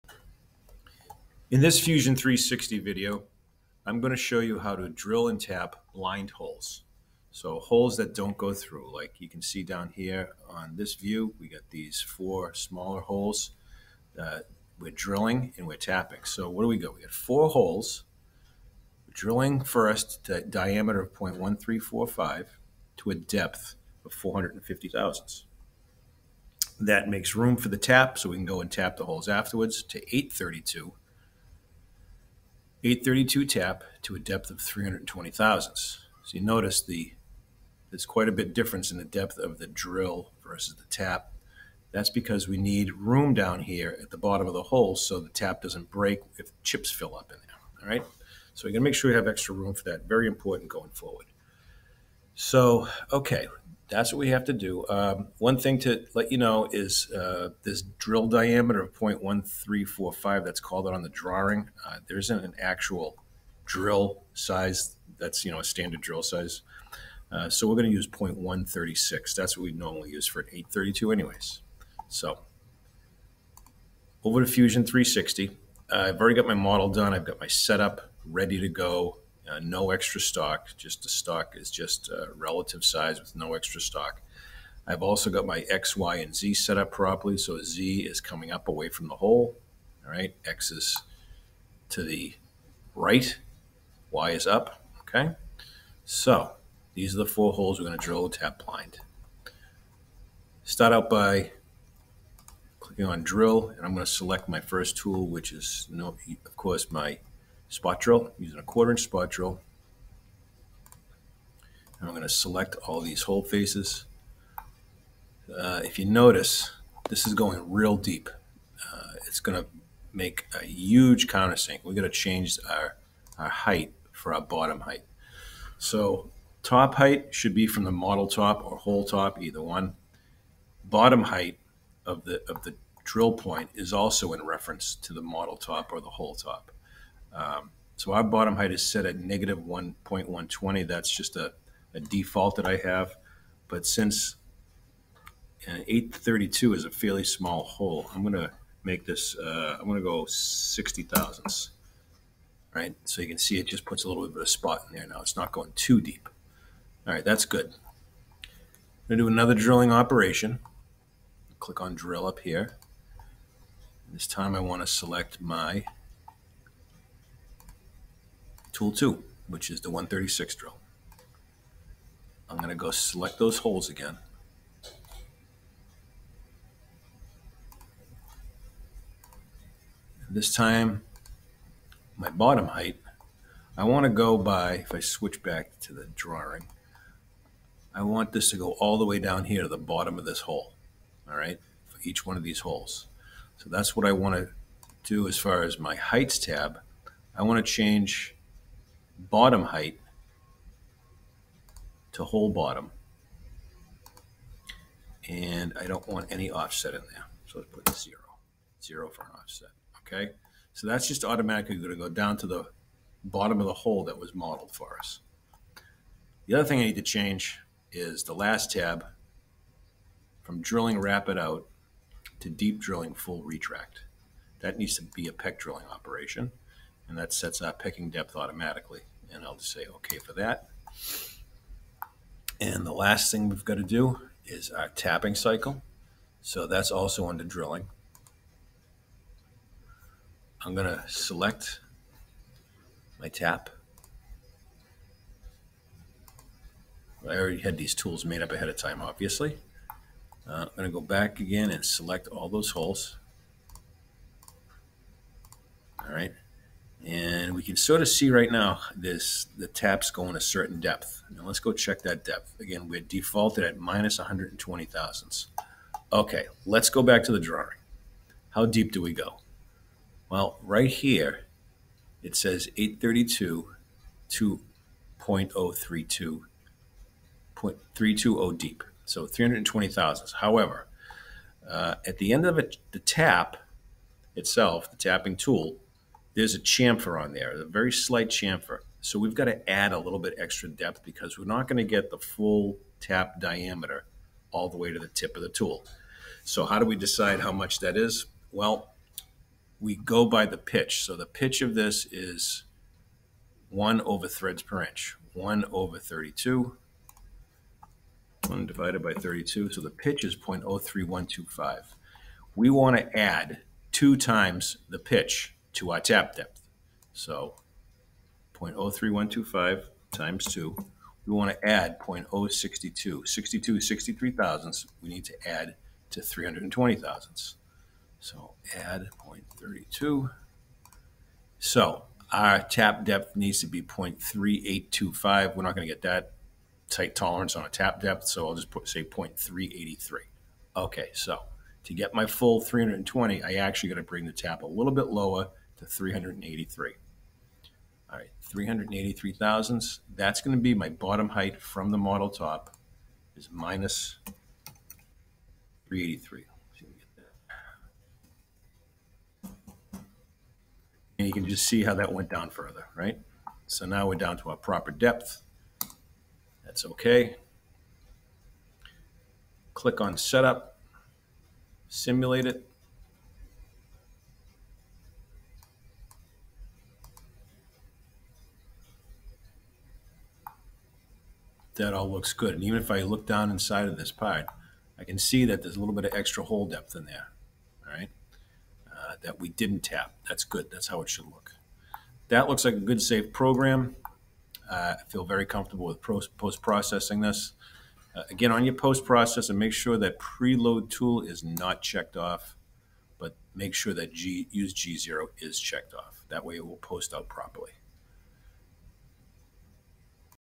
Drill and tap blind holes
Use Fusion 360 to drill and tap blind holes